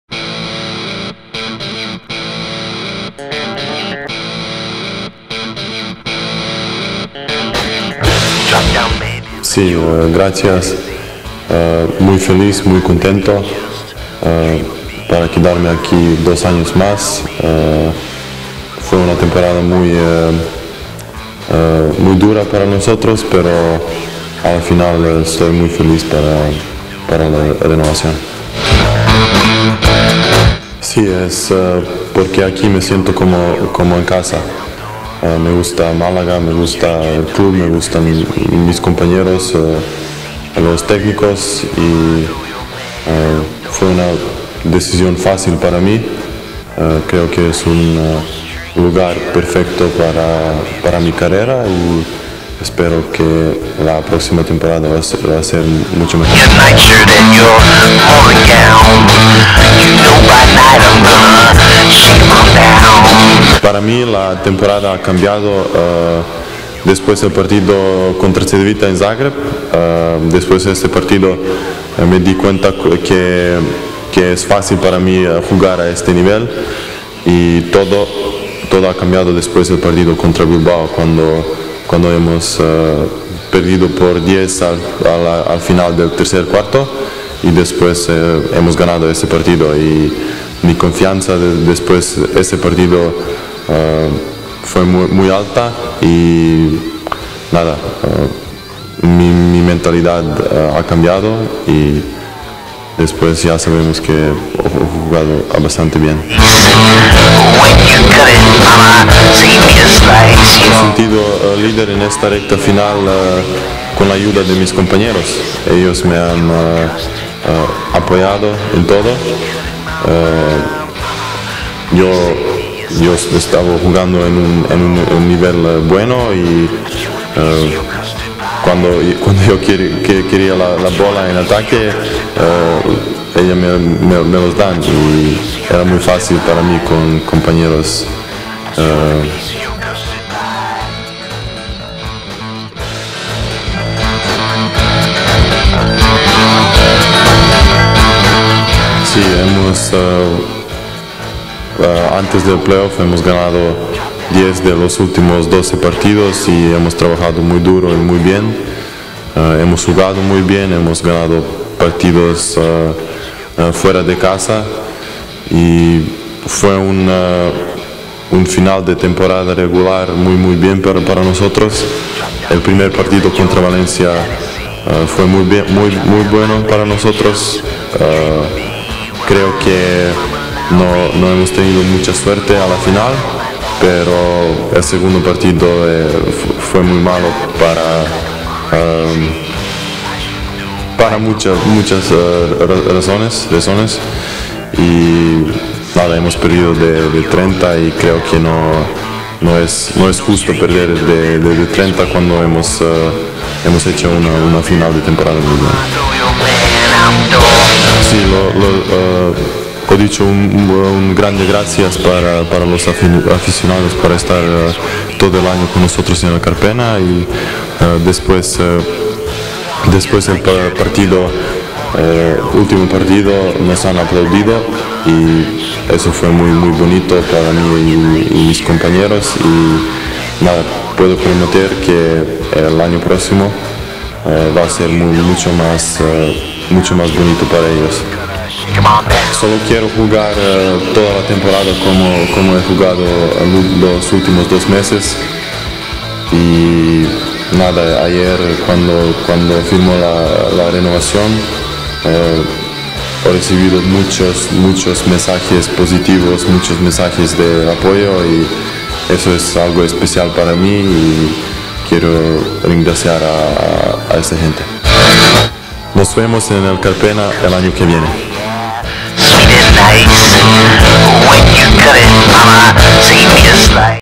Nedovic: "Encantado de seguir en Málaga, me siento como en casa"
Nemanja Nedovic estará dos años más en Málaga. El serbio ha querido agradecer al Club las facilidades para renovar, mostrándose ilusionado y con confianza para la próxima temporada. Además, ha agradecido que el equipo fuese despedido con una gran ovación por la afición, con la que se cita para el año que viene.